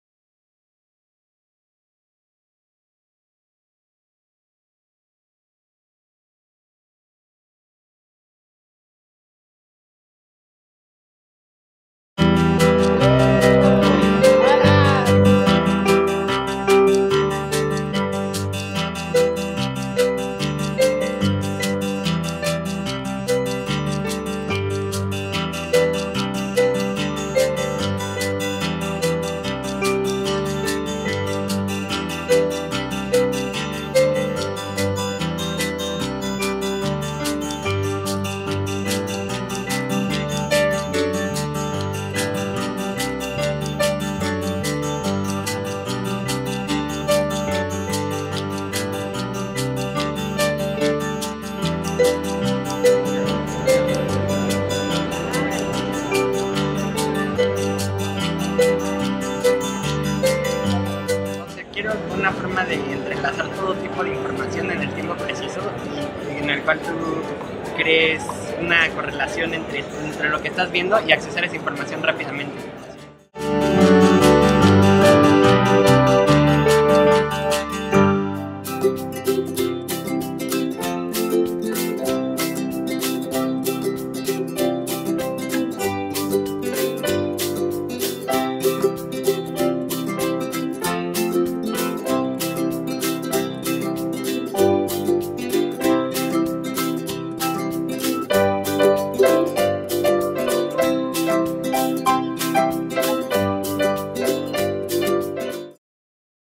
Blogueras en SVDCDMX
Blogueras vamos a Silicon Valley Day en la Ciudad de México, ¡increíble! Gracias Mujeres Construyendo por la invitación.